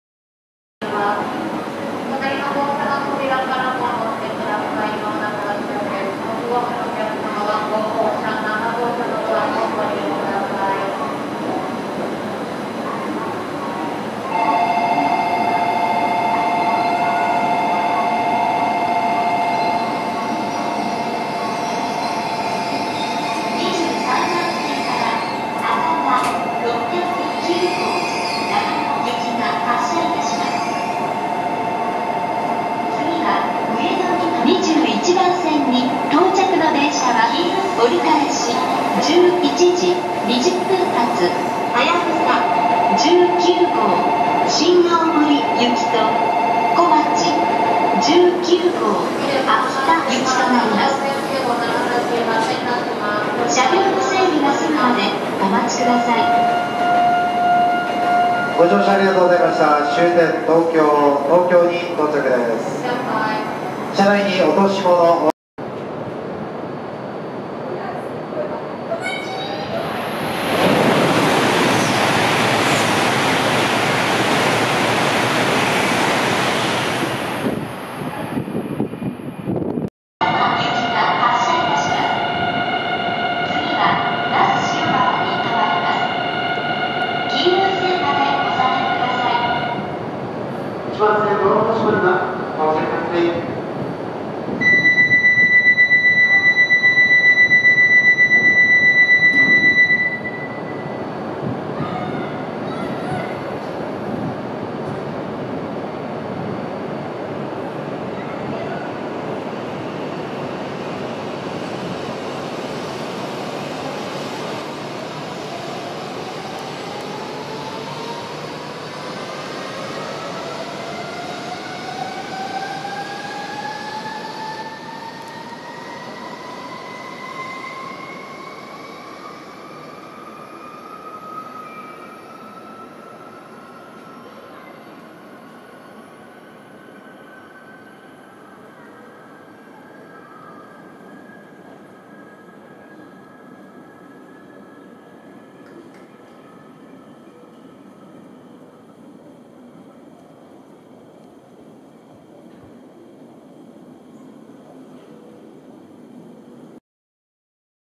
東北新幹線 東京駅と宇都宮駅発着 01.Feb.2020
東北新幹線発着を東京駅と宇都宮駅で撮影しました。
撮影日　2020年 2月 1日